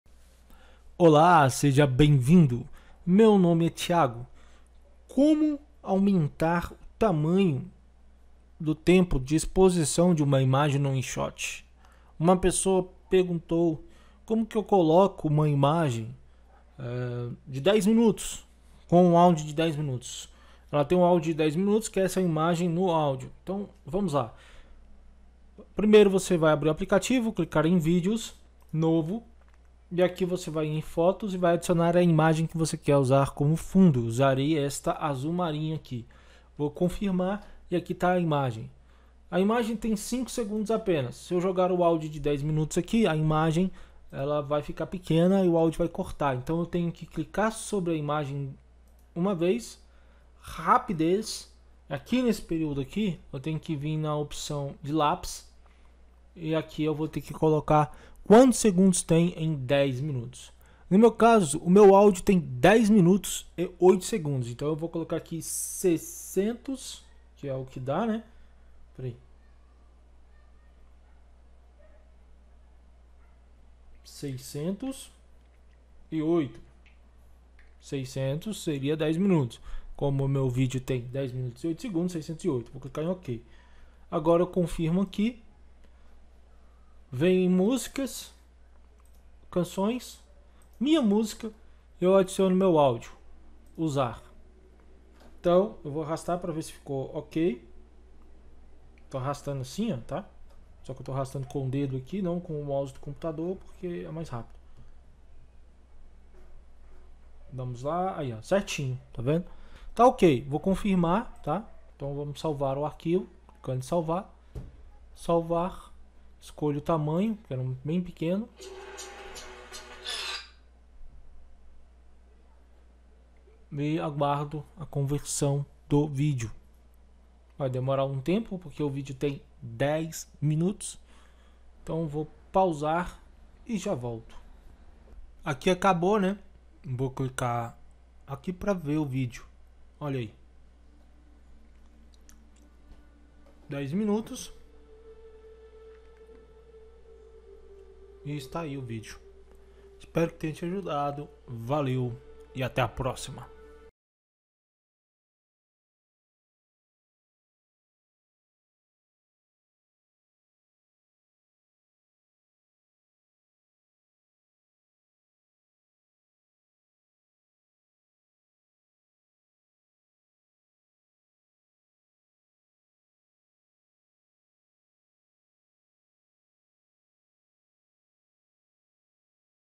Como aumentar o tempo de imagem no Inshot
Nᴇsᴛᴇ ᴠɪ́ᴅᴇᴏ ᴠᴏᴄᴇ̂ ᴠᴀɪ ᴀᴘʀᴇɴᴅᴇʀ ᴄᴏᴍᴏ Aᴜᴍᴇɴᴛᴀʀ ᴏ ᴛᴇᴍᴘᴏ ᴅᴇ ᴜᴍᴀ ɪᴍᴀɢᴇᴍ Nᴏ ɪɴsʜᴏᴛ

-𝙄𝙣𝙨𝙩𝙖𝙜𝙧𝙖𝙢 𝙊𝙛𝙛𝙞𝙘𝙞𝙖𝙡: 𝙩𝙝𝙞𝙖𝙜𝙤𝙙𝙚𝙥𝙤𝙡𝙤

►Ajude o canal com 1 real.